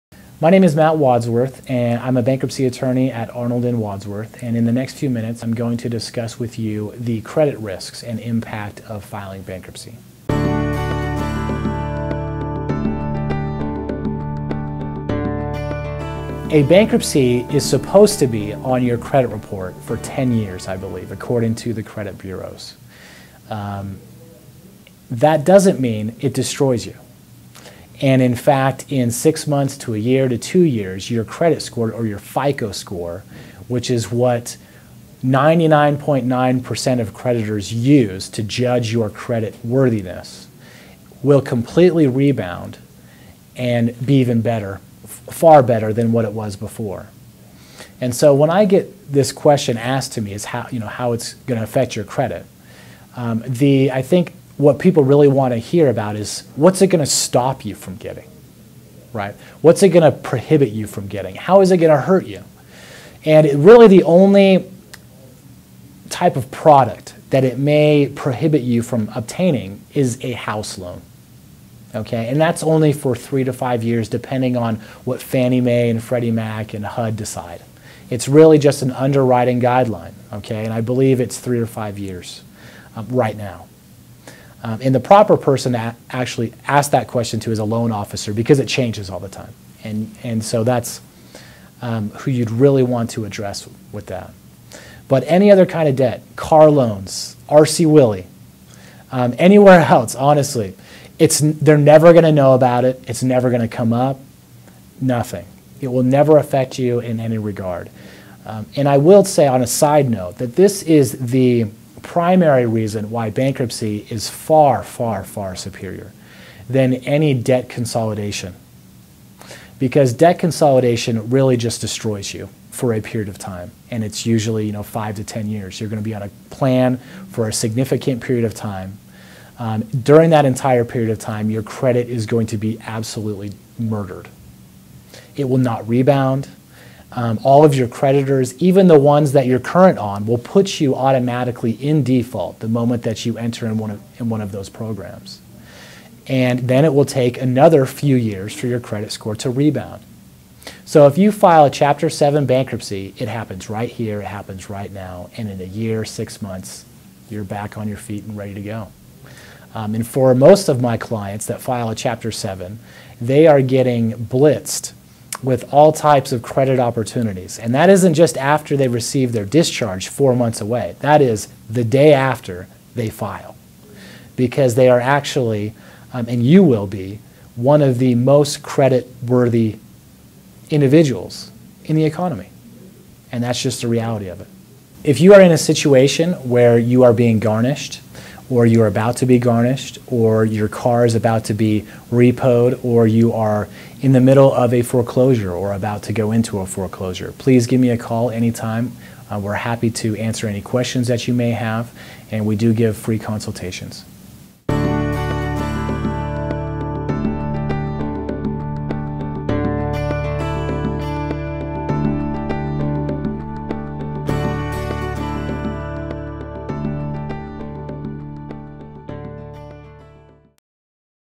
How Will Bankruptcy Affect My Credit? - Arnold & Wadsworth
"My name is Matt Wadsworth. I am a bankruptcy attorney at Arnold & Wadsworth and in the next few minutes I am going to discuss with you the credit risks and impact of filing bankruptcy. A bankruptcy is supposed to be on your credit report for 10 years I believe according to the credit bureaus. That doesn't mean it destroys you. And in fact in 6 months to a year to two years your credit report or FICO score which is what 99.9% of creditors use to judge your credit worthiness will completely rebound and be even better, far better than what it was before. And so when I get this question asked to me "How is it going to affect your credit"? I think what people really want to hear about is: "What's it going to stop you from getting? What's it going to prohibit you from getting? How is it going to hurt you"? And really the only type of product that it may prohibit you from obtaining is a house loan. And that's only for three to five years depending on what Fenny Mae and Freddie Mac decide. It is really just an underwriting guideline okay? And I believe it's just three or five year's right now. And the proper person to actually ask that question tois a loan officer because it changes all the time and so that's who you'd really want to address with that. But any kind of debt: car loans, RC Willey, anywhere else, honestly, they are never going to know about it. It is never going to come up, nothing. It will never affect you in any regard. And I would say on a side note that this is the primary reason why bankruptcy is far superior than any debt consolidation because debt consolidation really just destroys you for a period of time and it is usually for 5 to 10 years. You are going to be on a plan for a significant period of time. During that entire period of time your credit is going to be absolutely murdered. It will not rebound. All of your creditors, even the ones that you are current on, will put you automatically in default the moment you enter in one of those programs. Then it will take another few years for your credit score to rebound. So if you file Chapter 7 Bankruptcy it happens right here, it happens right now and in a year and six months you're back on your feet ready to go. And for most of my clients that file a Chapter 7, they are getting blitzed with all types of credit opportunities and that isn't just after they've receive their discharge four months away that is the day after they file because they actually, and you will be, one of the most credit worthy individuals in the economy. And that is just the realty of it. If you are in a situation where you are being garnished or you are about to be garnished, or your car is about to be repossessed, or you are in the middle of a foreclosure or about to go into a foreclosure. Please give me a call anytime. We are happy to answer any questions that you may have and we do give free consultations."

Arnold & Wadsworth Salt Lake City Office

298 24th Street
Suite #230